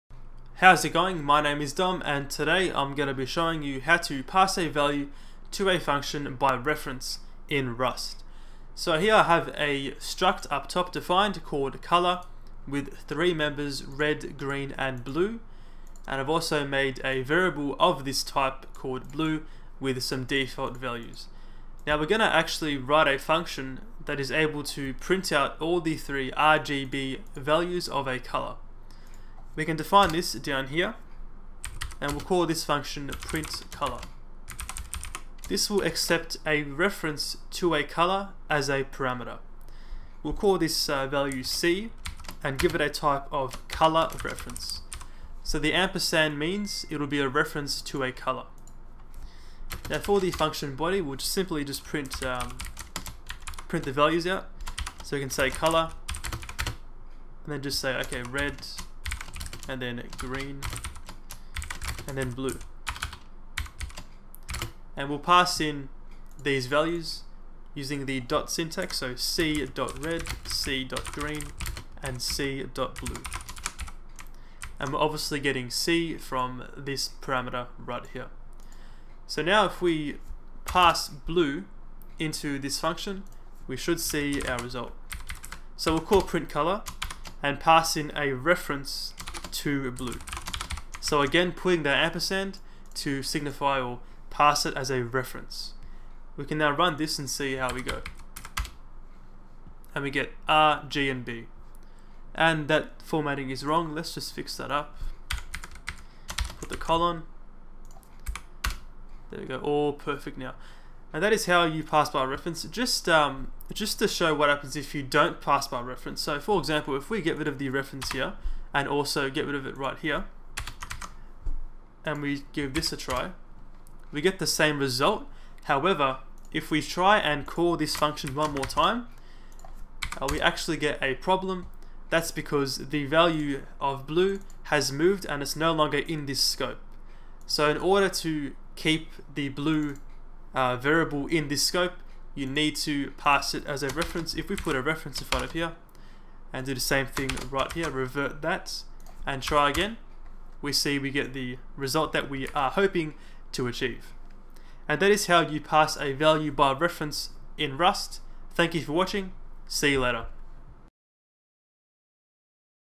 Rust Programming Tutorial #19 - Pass by Reference
In this video we take a look at how you can pass a value to a function by reference. This means you are borrowing ownership of the resource and it allows you to then continue using the variable once the function call has completed.

The example shows a struct 'Color' with three members: red, green and blue. We then create a function 'print_color' that prints out the values to the console, accepting a reference to a Color as an argument.